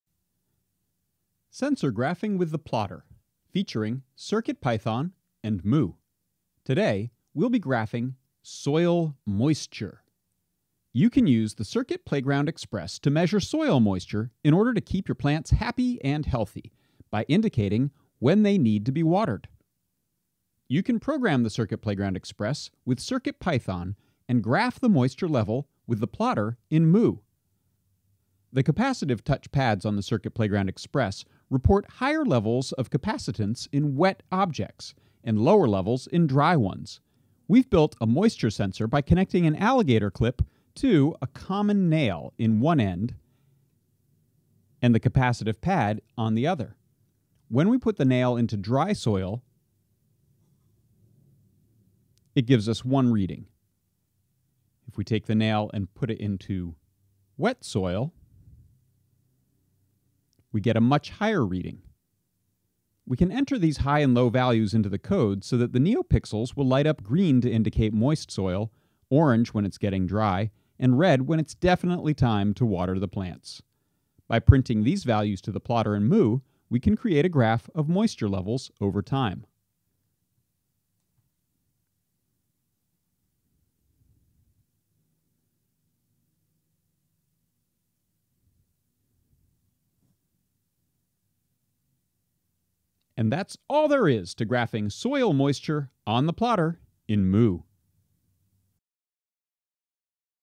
Sensor Graphing with the Plotter: Soil Moisture @adafruit @johnedgarpark
Plot soil moisture in Mu's plotter graph with Circuit Playground Express! Coded with CircuitPython.